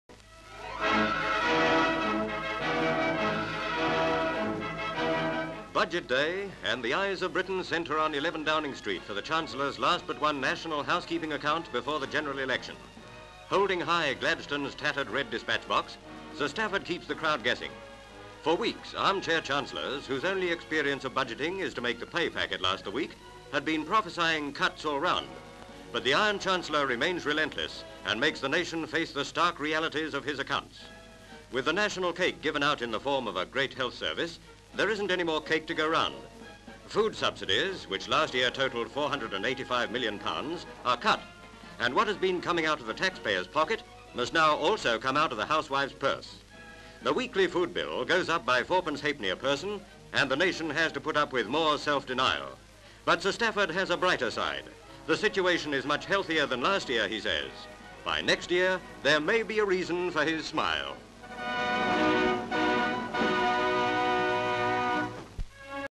Spending Review Furore 1949 (1949)
Full title reads: "Cripps Cuts His 'No Cake' Budget".

London.

LV Crowds waiting in Downing Street. CU 2 women in crowd, One holding shopping bag. CU elderly man. CU No 11 Downing St. door. SV Photographers pan to Sir Stafford Cripps, Chancellor of the Exchequer coming out of No 11. CU 2 women looking on. CU Budget box pan up to Sir Stafford. CU Sir Stafford. CU Budget case pan up to Sir Stafford laughing. SV Crowd. SV Sir Stafford's car drives away.

SV Big Ben (Clock tower of Palace of Westminster) showing 20 to 6 pan down to people buying papers. CU Placard - "Special Budget Edition." SV People buying papers. CU Female type pan down to newspaper with headline 'Grim & Bear It Budget."

Chapel St. Market, London.

SV One person's ration in centre of butcher's shop window. CU Meat ration. SV People looking in grocer's shop at cheese, butter and margarine. CU Cheese pan to margarine & butter. CU Woman arguing. CU Man arguing. SV Group arguing. CU Woman arguing. CU Another woman arguing.

CU Sir Stafford with a big smile.
 FILM ID:1409.3